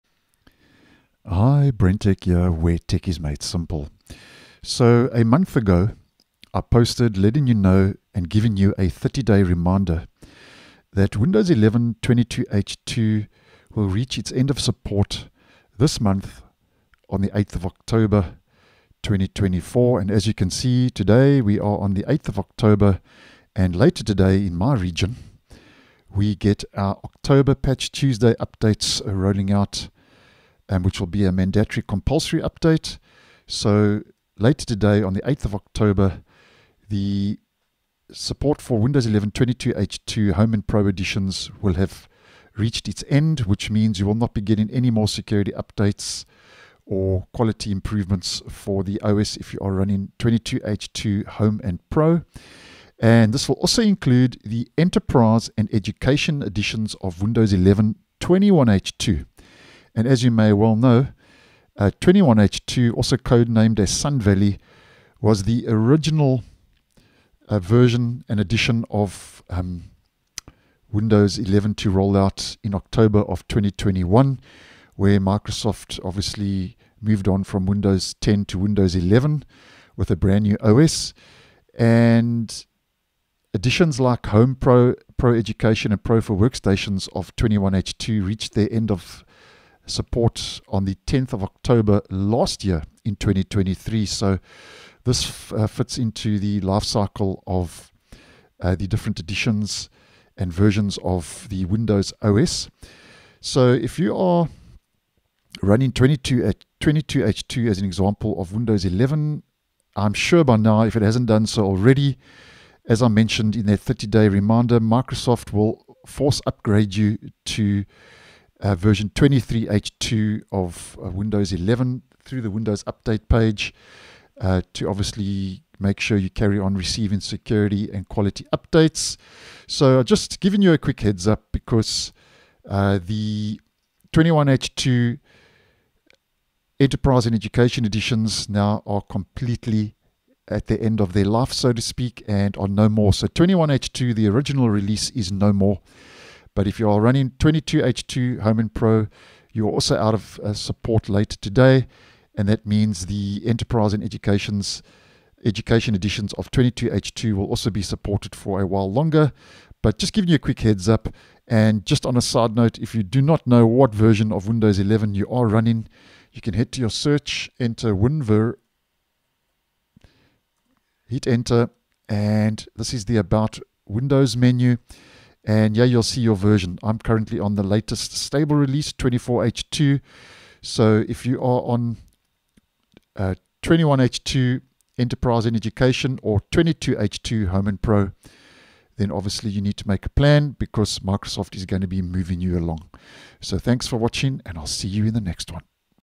Today Oct 8, 2024 Windows 11 22H2 is Out of Support & Windows 11 21H2 Reaches End of Life
This includes the Home and Pro editions of Windows 11 22H2. The original release of Windows 11 has now also completely reached the end of the road with the end of support of the Enterprise and Education editions of 21H2.